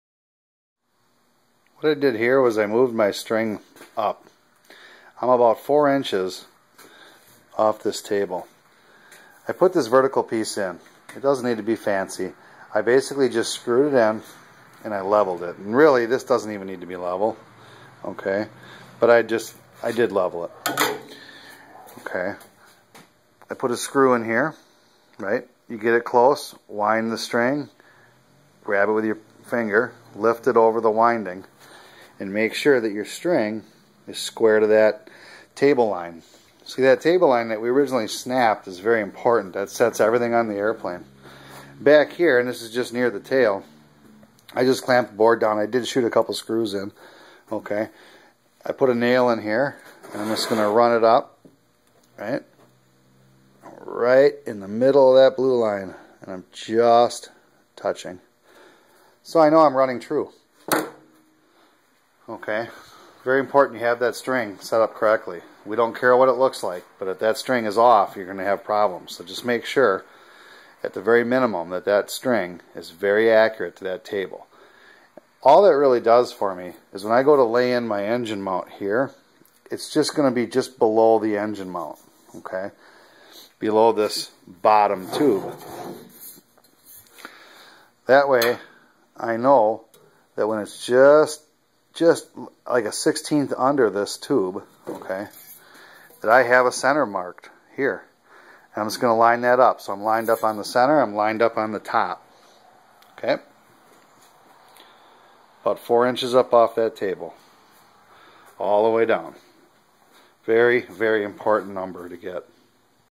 Legal Eagle Ultralight construction video #51
Firewall assembly for the Legal Eagle ultralight airplane